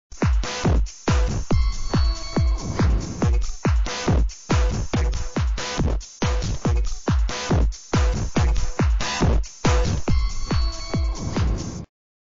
Our new Intro
The new intro made by Mark and James.
Mark did the video and James did the music.